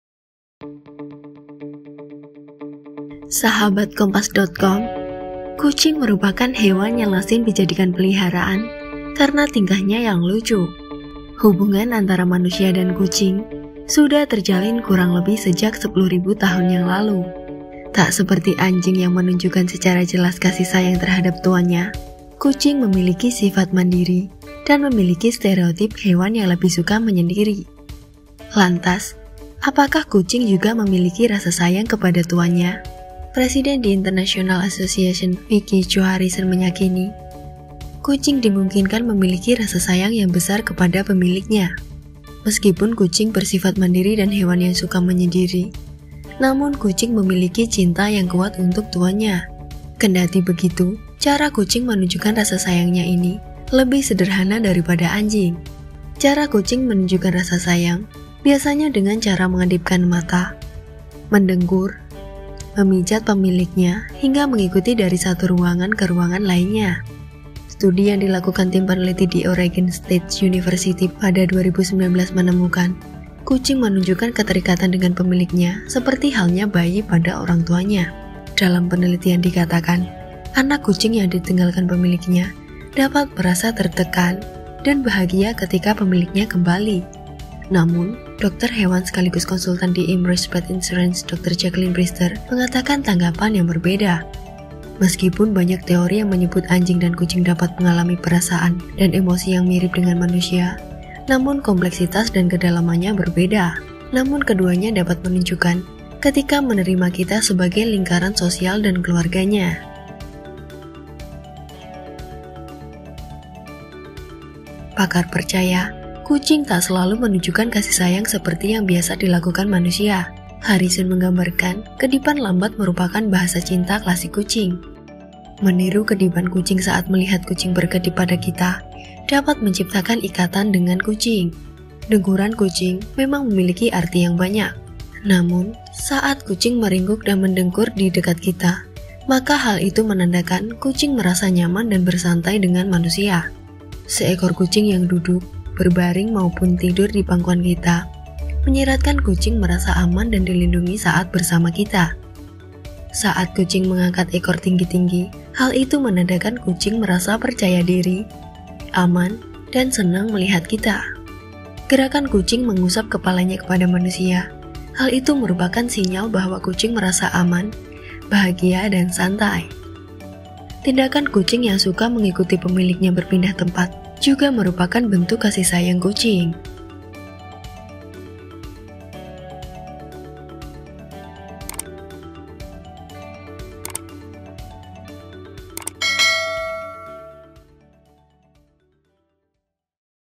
Apakah Kucing Dapat Memiliki Rasa Sayang kepada Pemiliknya?
Hubungan antara manusia dan kucing sudah terjalin sejak lebih kurang 10.000 tahun yang lalu. Tak sedikit dari kita yang memelihara seekor kucing karena merasa gemas dengan tingkah laku hewan berkumis itu. Apakah Anda penasaran, kucing sebenarnya menyayangi kita atau tidak? Sebab, kucing bukanlah anjing yang secara jelas menunjukkan kasih sayang terhadap tuannya. Simak selengkapnya dalam video berikut! Penulis: Gading Perkasa Penulis Naskah: Meiva Jufarani Narator: Meiva Jufarani Editor: Armitha Sathi Devi Produser: Deta Putri Setyanto